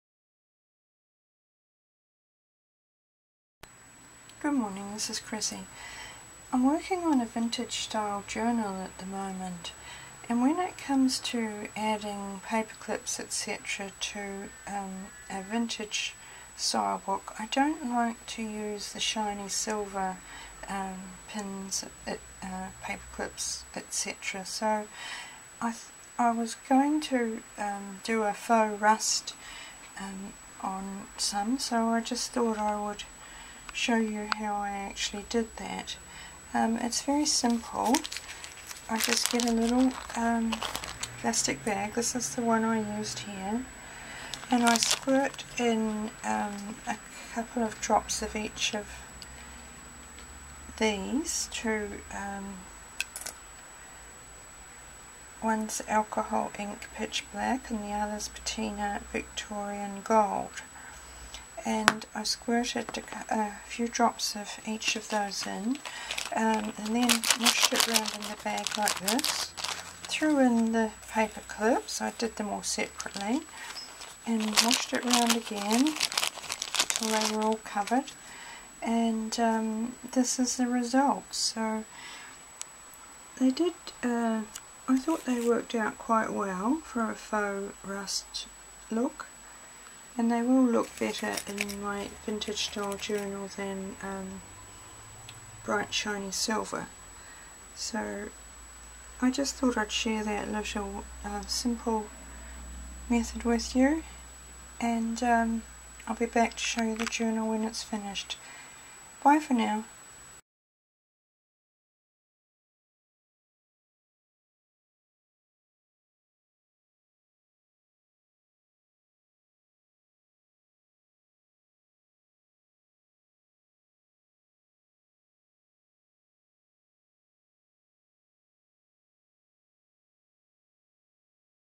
'Faux Rust'
I 'faux rusted' some bits for my vintage style journals and this is how I did it.